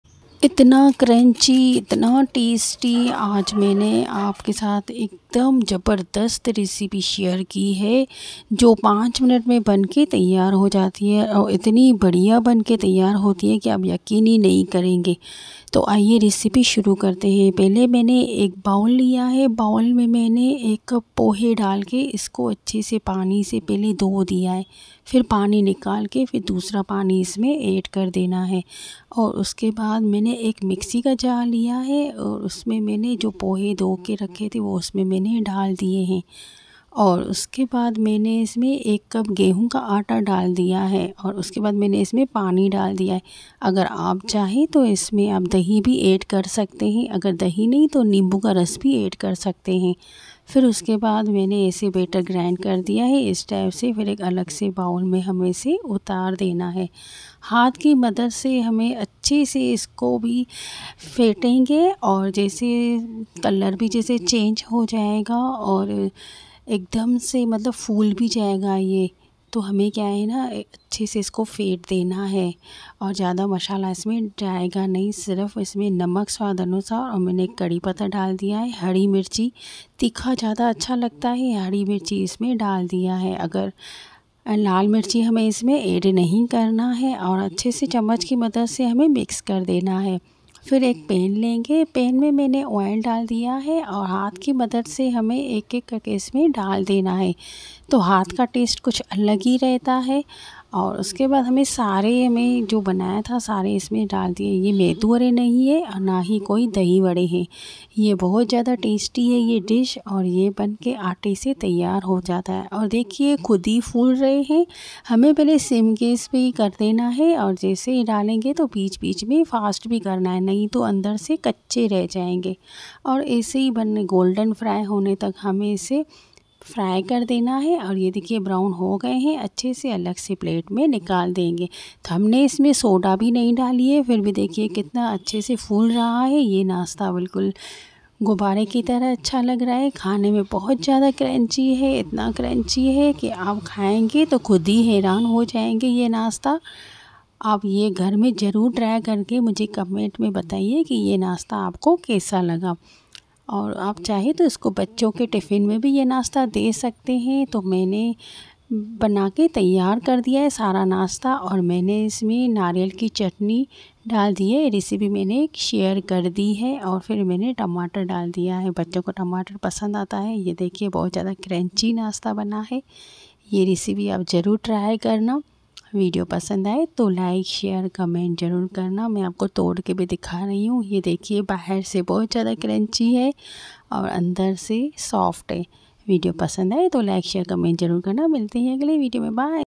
5 मिनट में पोहा का ऐसा कुरकुरा टेस्टी नाश्ता बनाया जिसने खाया उसको बड़ा पसंद आया | Poha Recipe
5 मिनट में पोहा का ऐसा कुरकुरा टेस्टी नाश्ता बनाया जिसने खाया उसको बड़ा पसंद आया | Poha Recipe

POHA SNACKS